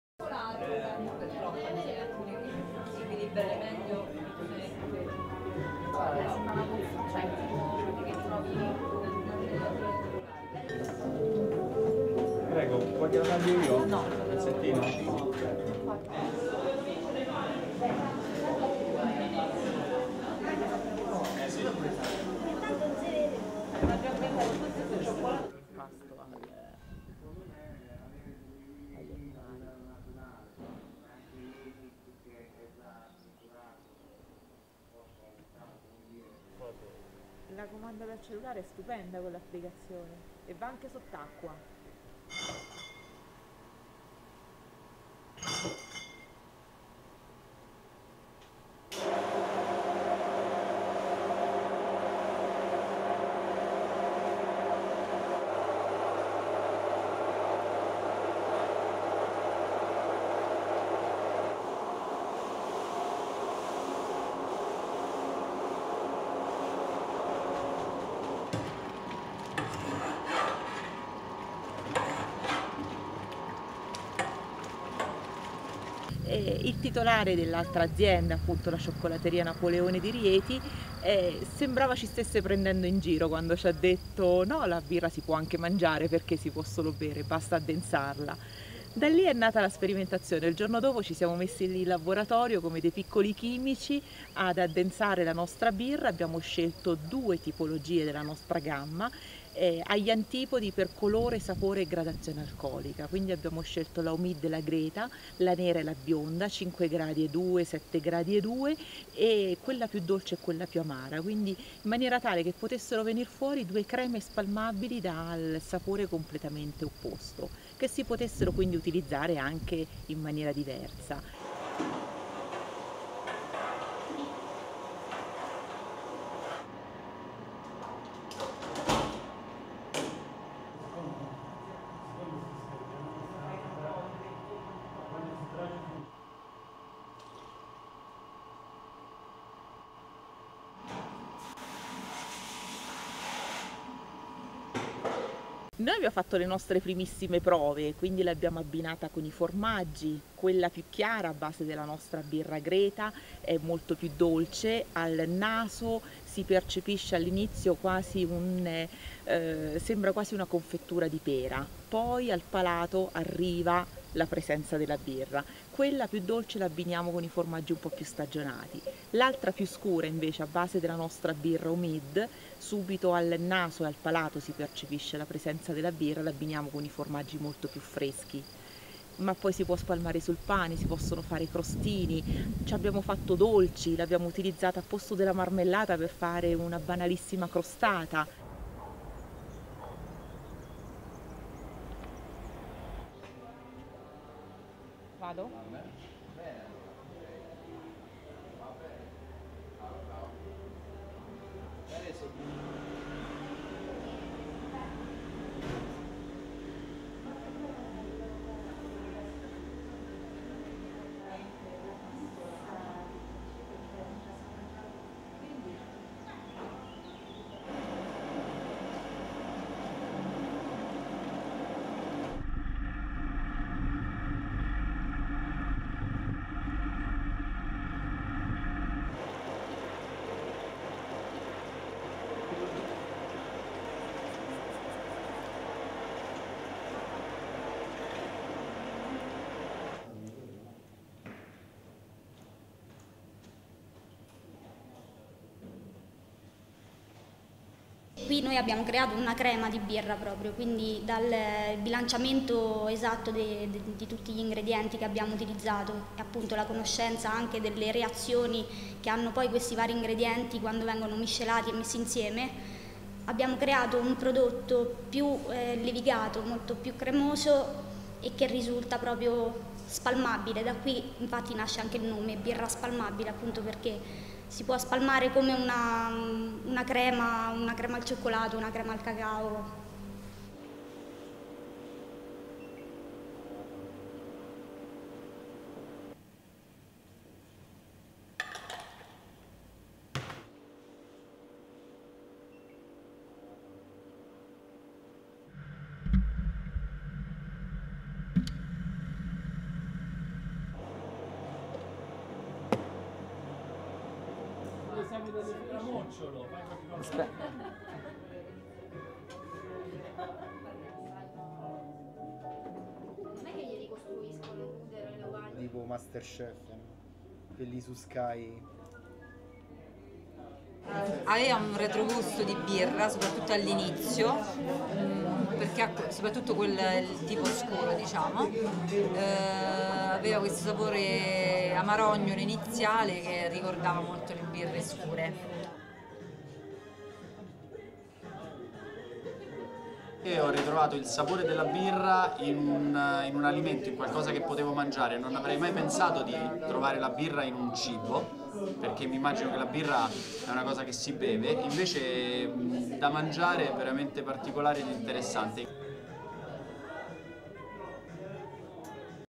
The latest culinary delight - spreadable beer
Anguillara Sabazia, Rome - July 7, 2013
1. Mid of pub customers drinking a beer
2. Mid of bartender pouring a beer
3. Close up of a glass of beer
Rieti -  July 7, 2013
4. Close up of tray with appetisers made with spreadable beer
5. Close up of topping an ice cream with spreadable beer
Cittareale, Rieti -  July 7, 2013
6. Pan right from mountains to the "Alta Quota" micro-brewery's building
7. Wide of  "Alta Quota" entrance with a herd of sheep in the background
8. Interior wide of "Alta Quota" micro-brewery's warehouse
9. Mid of "Alta Quota" brewer crushing malt
10. Close up of malt in the crushing machine
11. Mid of "Alta Quota" brewer pouring crushed malt into hot water inside a mash tun
12. Close up of mixing crushed malt with hot water
13. SOUNDBITE : (Italian) Emanuela Laurenzi, co-owner of "Alta Quota" micro-brewery:
"It looked like Pietro Napoleone, the owner of the chocolate shop, was making fun of us telling us that it would be possible to eat beer and not only drink it and that it was just a matter of thickening it. Since then we started our experiments to thicken it, just like little chemists and we choose two types of our (beer) selection which are very different in terms of colour, flavour and alcohol strength: therefore we've chosen "Omid" and "Greta", a dark one and a pale one,  seven point two degrees and five point two degrees (alcohol content), the most bitter one and the sweetest one to obtain spreadable creams, tasting each completely different from the other, to use them in a different way."
14. Wide of brewer mixing crushed malt with hot water
15. Mid of brewer closing the mash tun
16. Close up of filtering fermentable liquid from the grains
17. Mid of brewer checking fermentable liquid
18. Close up of screen showing parameters for beer preparation
19. SOUNDBITE : (Italian) Emanuela Laurenzi, co-owner of "Alta Quota" micro-brewery:
"We did our very first tests by coupling it with cheeses. The pale one, made with our beer "Greta" is much sweeter and at the very beginning is possible to smell a flavour of pear in it and then the palate would taste the presence of a real beer flavour; this one couples best with seasoned cheeses. The other one, which is darker, is based on our beer Omid and immediately we can smell and taste an intense beer flavour and we couple it with very fresh cheese. But then is possible to spread on bread, to make croutons with it: we've also made cakes with it or we used it instead of marmalade to make an ordinary tart.
20. Wide of Rieti
21. Pan right from "Napoleone" chocolate shop's sign to entrance
22. Pan right of interiors of the chocolate shop
23. Pull focus of sign reading: "World Chocolate Master - Italian Championship of Chocolate Makers - Judge Pietro Napoleone".
24. Mid of chocolate lab worker mixing beer with thickeners to make the spreadable beer
25. Close up of the mixing process
26. Mid of chocolate lab worker collecting beer mixed with thickeners
27. Close up of chocolate lab worker collecting beer mixed with thickeners
28. Zoom out of chocolate lab worker starting the final thickening process
29. SOUNDBITE : (Italian) Francesca Napoleone, Commercial Manager of "Napoleone" chocolate makers :
"Here we created a cream made of beer by balancing exactly every ingredient we used and also by knowing the reactions are made by mixing these ingredients: so we've created a product which is very smooth and creamy, resulting, as the name itself suggests, in a spreadable beer because it can be spread just like a chocolate cream."
Anguillara Sabazia -  July 7, 2013